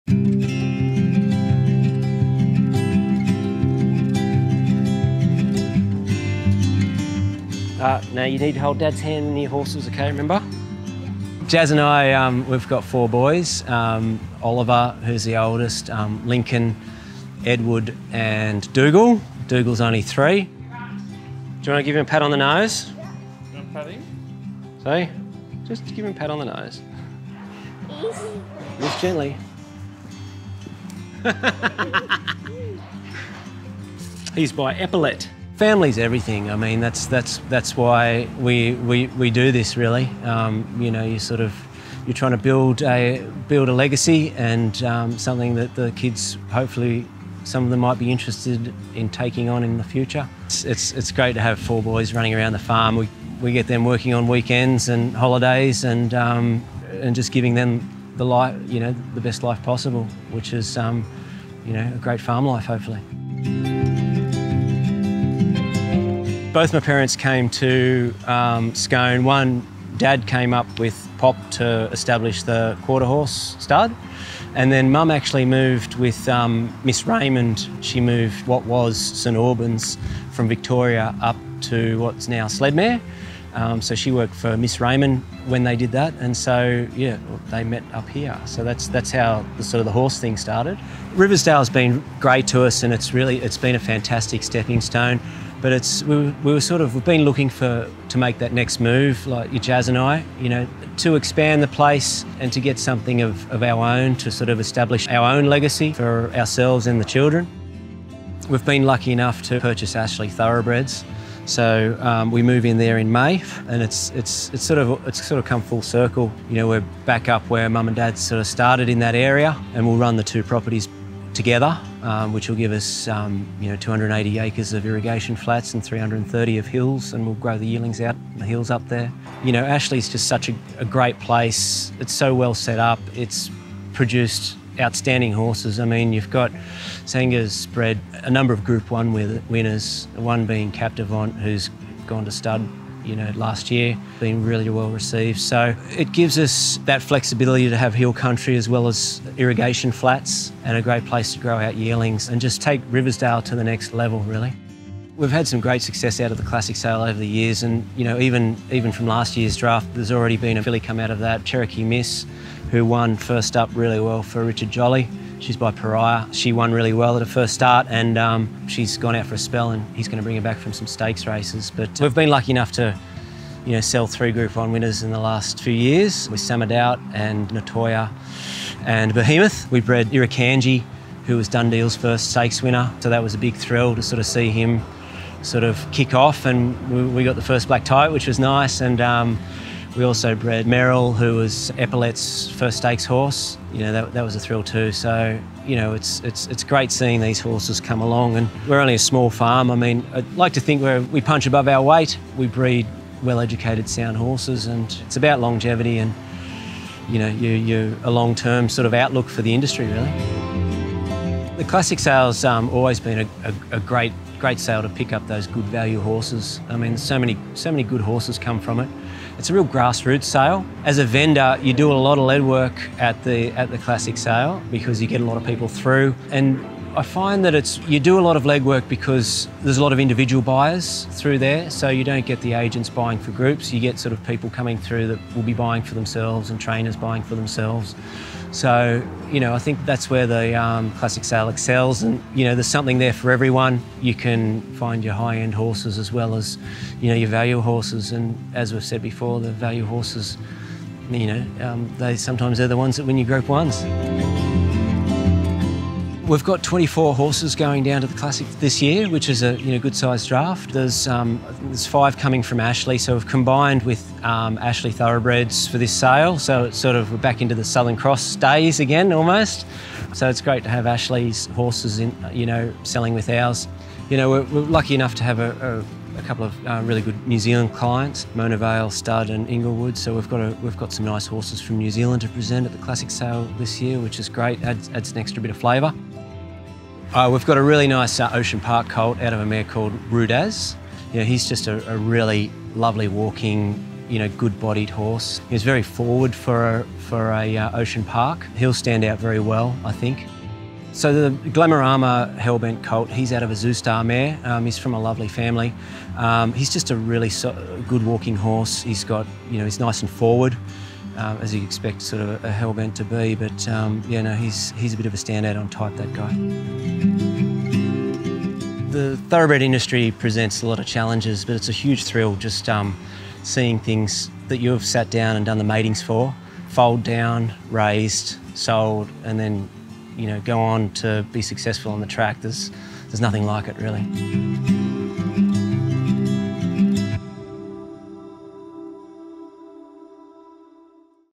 Riversdale: vendor preview - 2023 Inglis Classic Sale
It’s always been ‘family first’ for Nick and Jasmine Hodges, who are kept busy raising their four boys as well as managing their Riversdale property – which has been in the Hodges family for close to 40 years - just outside of Scone.

But it will soon be expanding, with Nick and Jasmine recently purchasing Senga Bissett’s Ashleigh Thoroughbreds, located nearby on Cressfield Road.

It’s an exciting time of growth for their operation, which has already provided G1-winning Inglis graduates Samadoubt and Nettoyer and regularly “punches above its weight.”

Riversdale will offer a draft of 24 yearlings – including five from Ashleigh Thoroughbreds – at InglisClassic, which starts Sunday Feb 12.